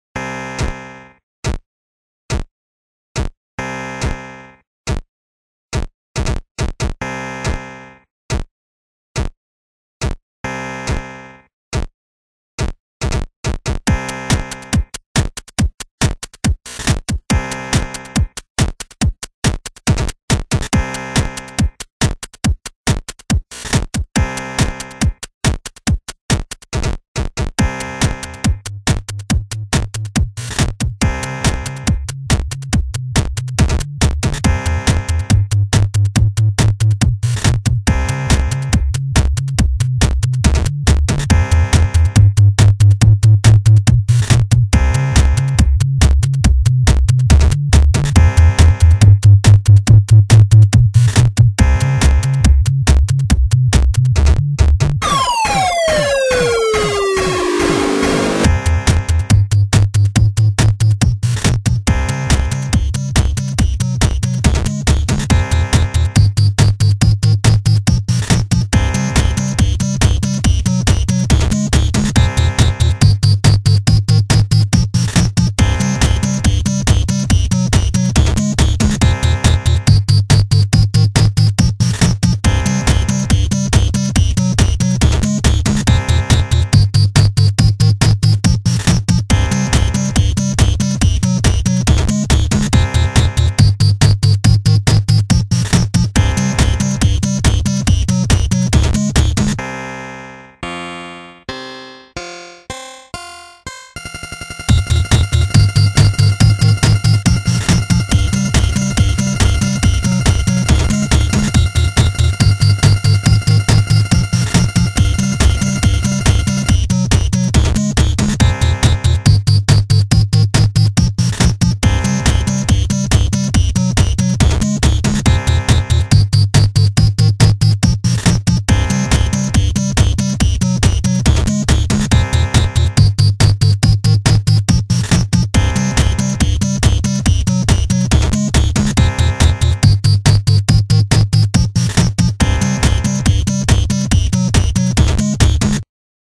dnb 8 bit try - Spidobot
trying to make dnb with 8 bit samples. i'm working on it.